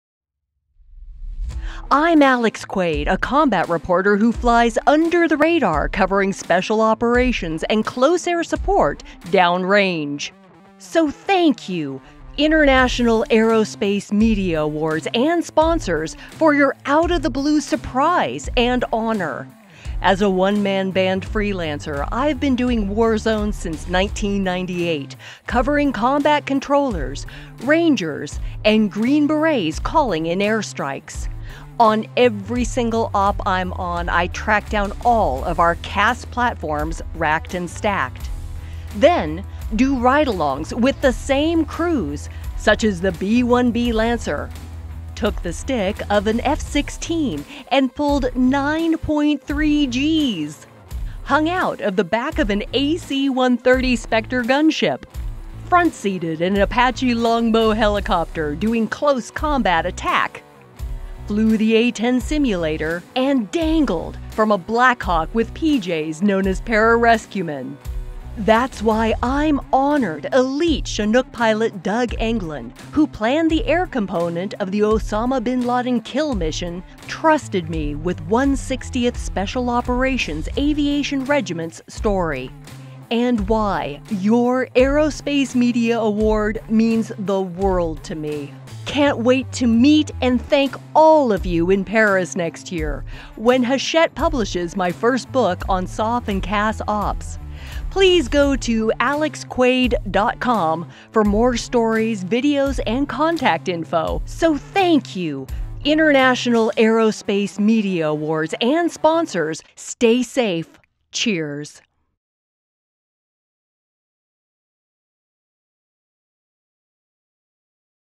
Alex Quade's Aerospace Award Acceptance
War reporter Alex Quade covers SOF & CAS operations.  Here's her video acceptance speech for Aerospace Award, for her exclusive: "Legendary Special Operations Aviator Reveals Bin Laden Mission Details For The First Time."